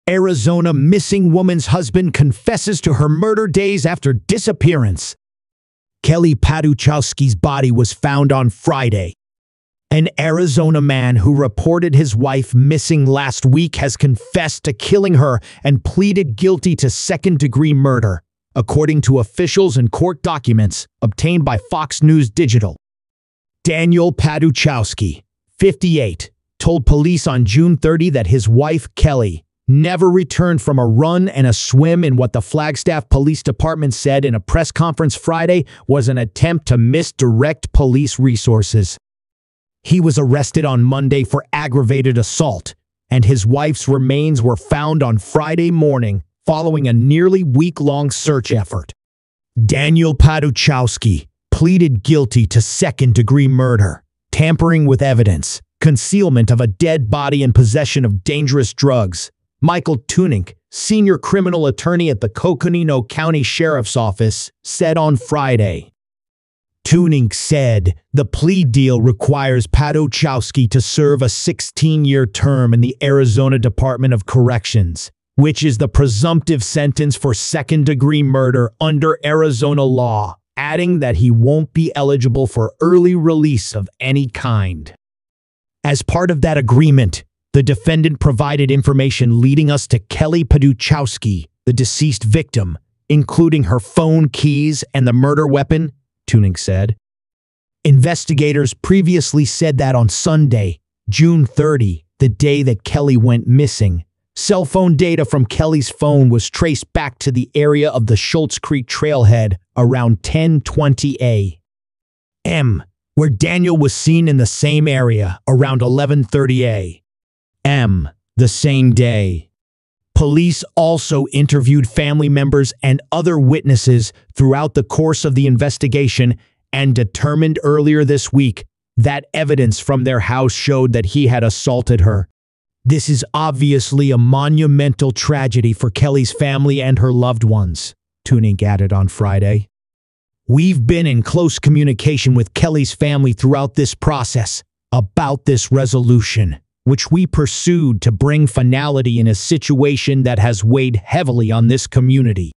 Arizona missing woman's husband confesses to her murder days after disappearance
Kelly Paduchowski's body was found on Friday
An Arizona man who reported his wife missing last week has confessed to killing her and pleaded guilty to second-degree murder, according to officials and court documents obtained by Fox News Digital.

Daniel Paduchowski, 58, told police on June 30 that his wife, Kelly, never returned from a run and a swim in what the Flagstaff Police Department said in a press conference Friday was an "attempt to misdirect police resources."

He was arrested on Monday for aggravated assault, and his wife’s remains were found on Friday morning following a nearly week-long search effort.

"Daniel Paduchowski pleaded guilty to second-degree murder, tampering with evidence, concealment of a dead body and possession of dangerous drugs," Michael Tunink, senior criminal attorney at the Coconino County Sheriff’s Office said on Friday.

U.S. NEWS
America
USA
United States
US politics
American culture